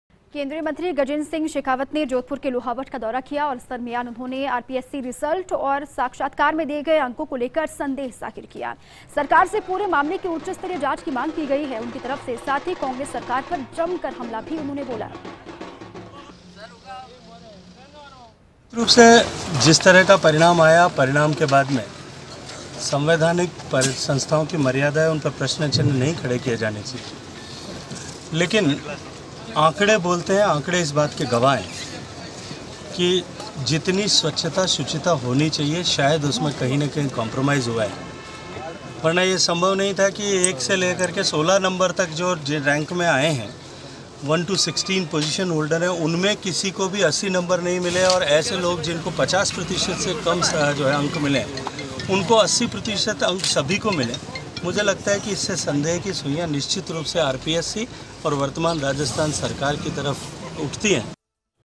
Gajendra Singh Shekhawat ने RAS-2018 के साक्षात्कार के अंकों पर जताया संदेह
About this Video:
Gajendra Singh Shekhawat casts doubt on RAS-2018 interview scores